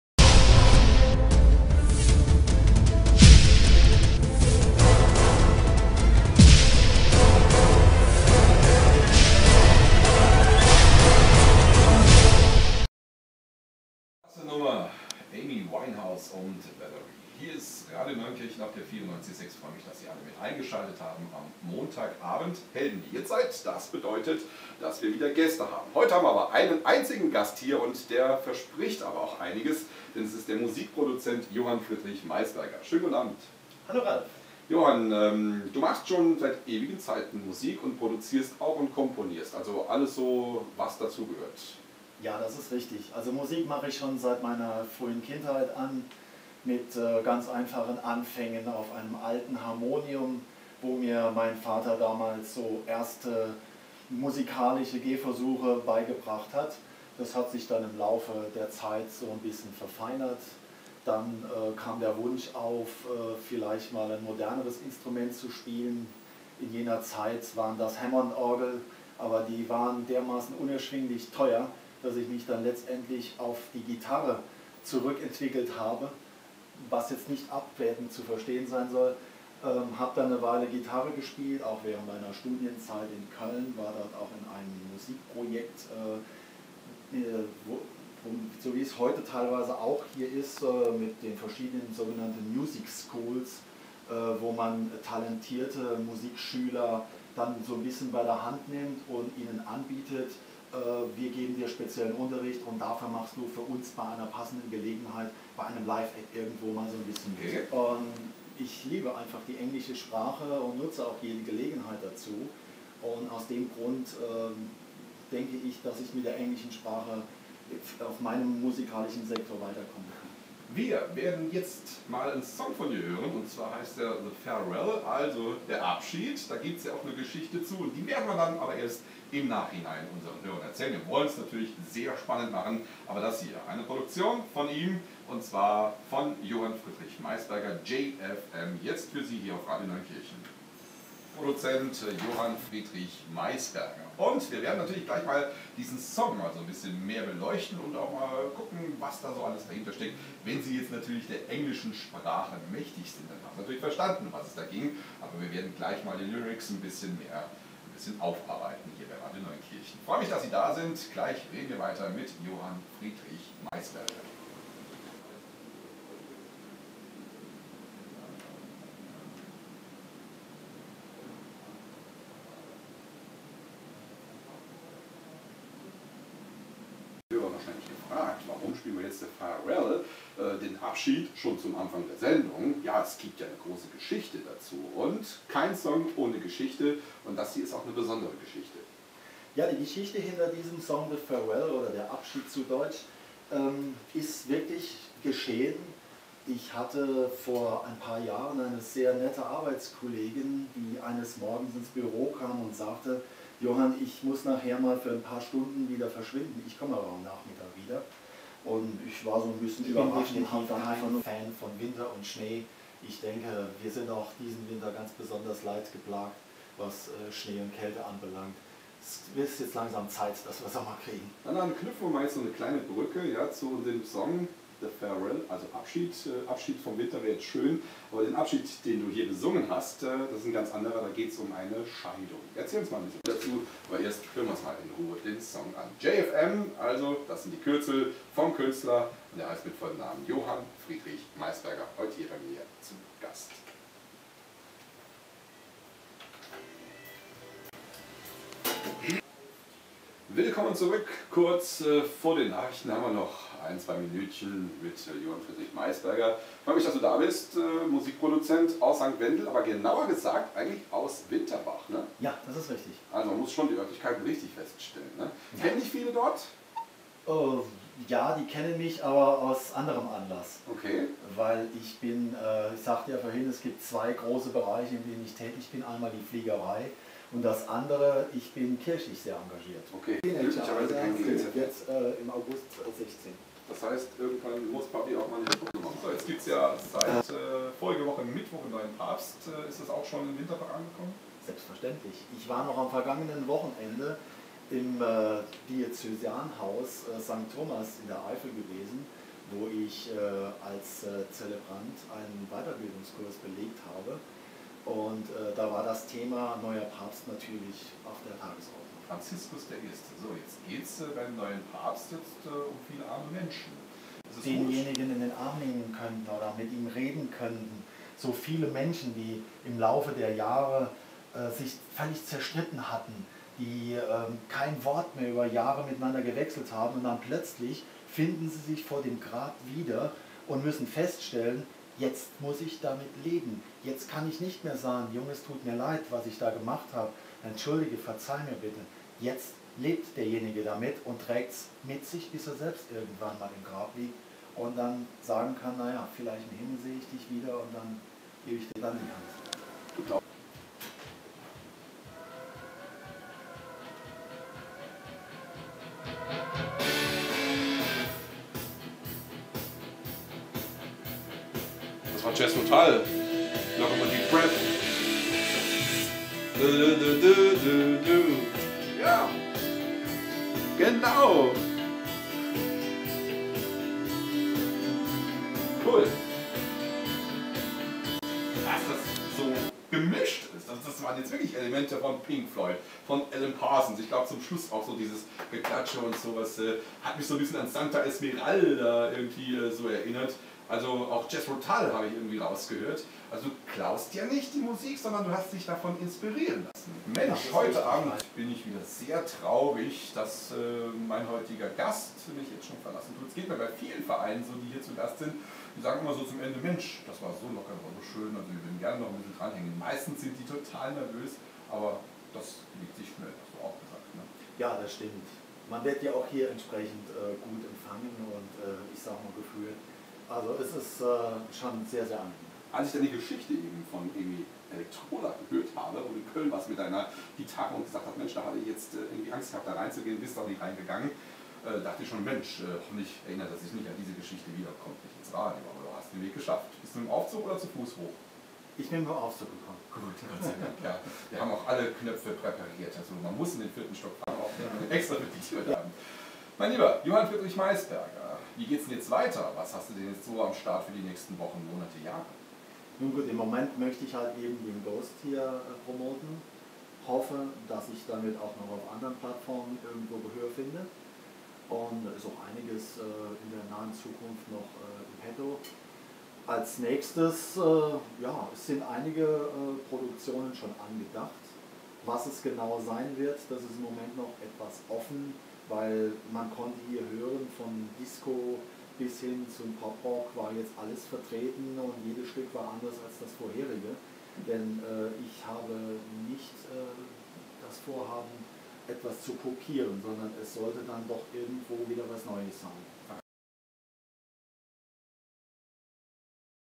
Zu Gast bei Radio Neunkirchen 94.6 ToMeWi Musikproduktion
Johann Friedrich Meisberger zu Gast bei Ralf Thiel am 18. März 2013.
Erstellt von Sportvertrieb-hasselberg / Ralf Hasselberg